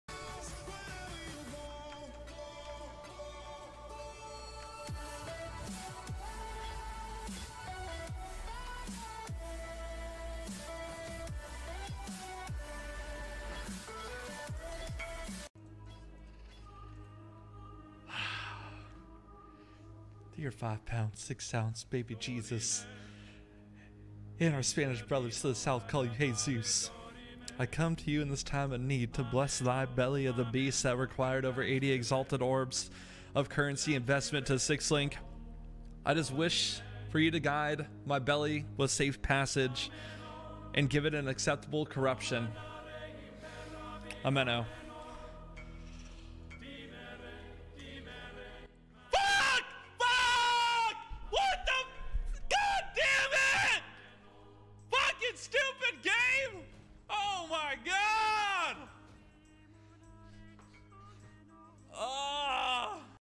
Datmodz spends 6000+ fusings on a chest trying to link it then corrupts it
THE CHEST IS NOT WORTH 80 EXALTS BUT HE HAS SPENT THAT IN FUSINGS SIX LINKING IT